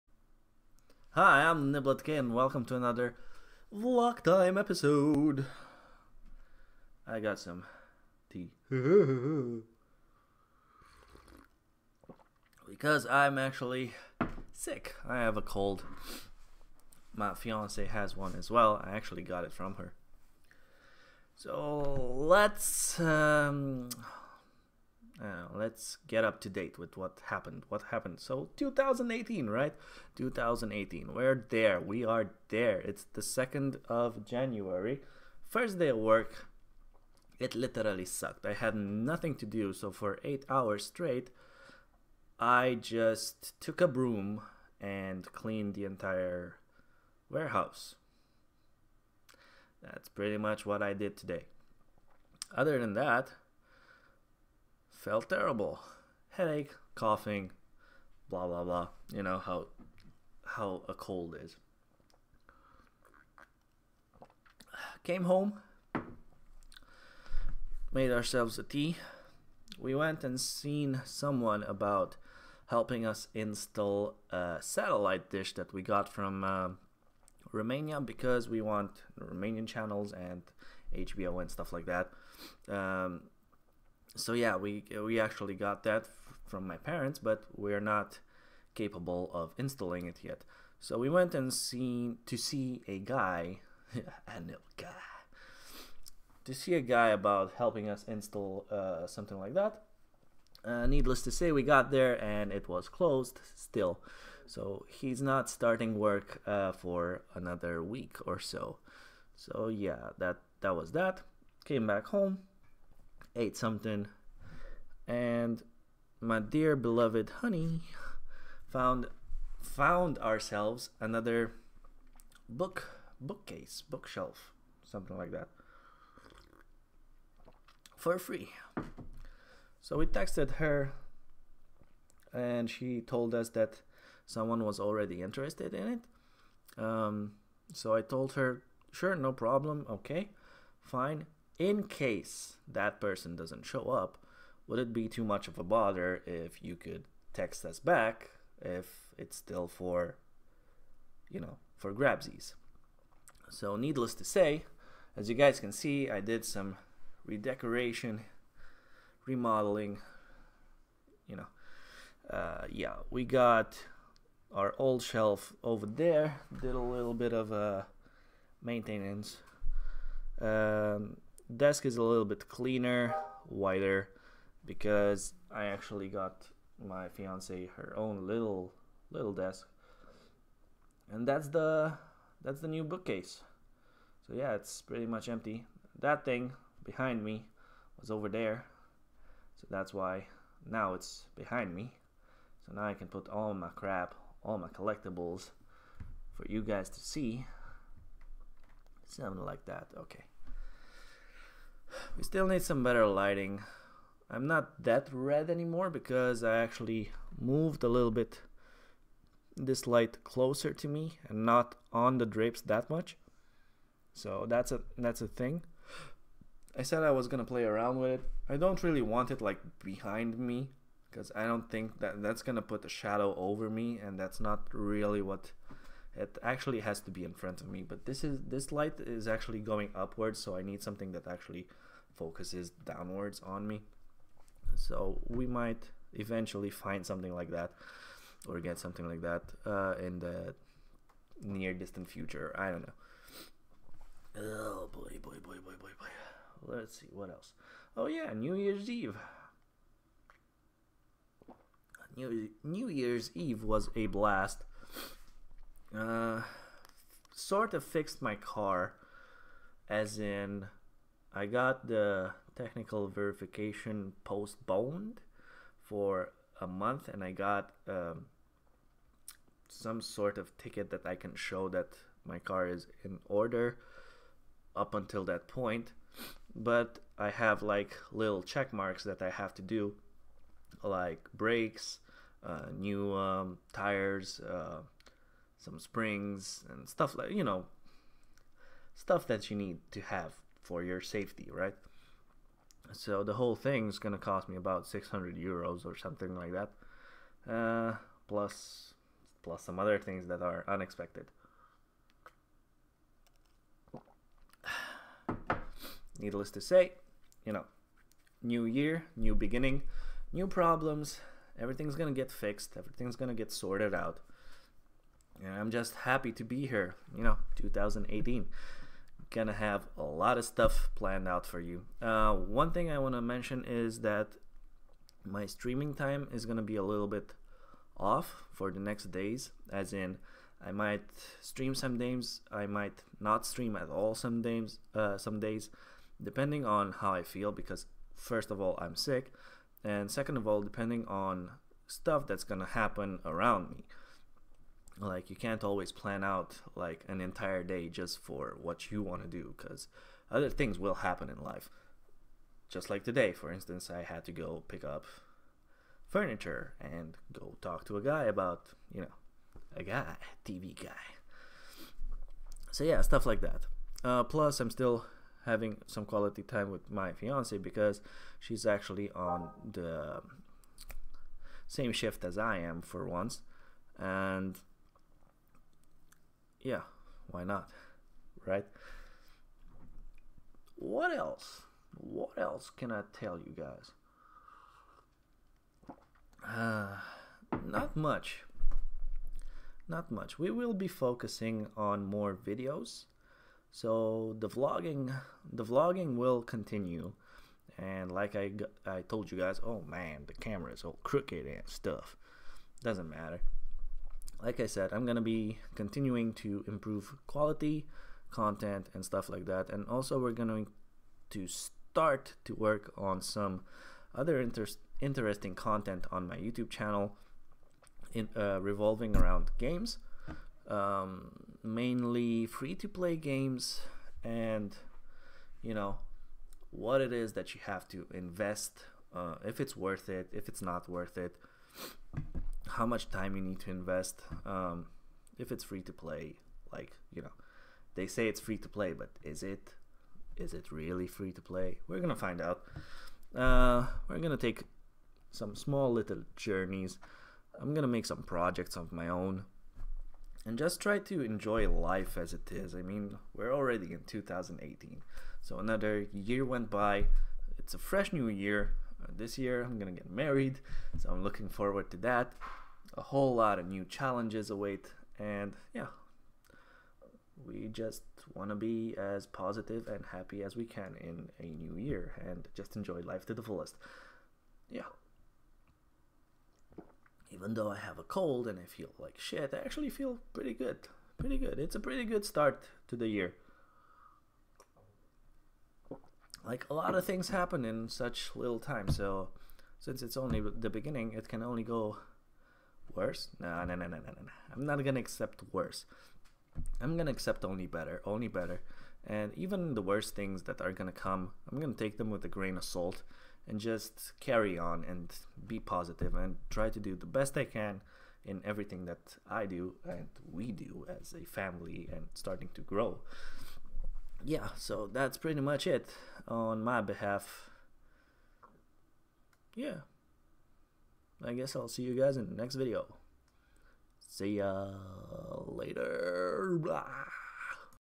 Vlogtime 02.01.18 Looking forward to the Future , #2018
Getting sick at the start of the year , but no worries , all is good. Looking forward to a brand new year , 2018 is my year.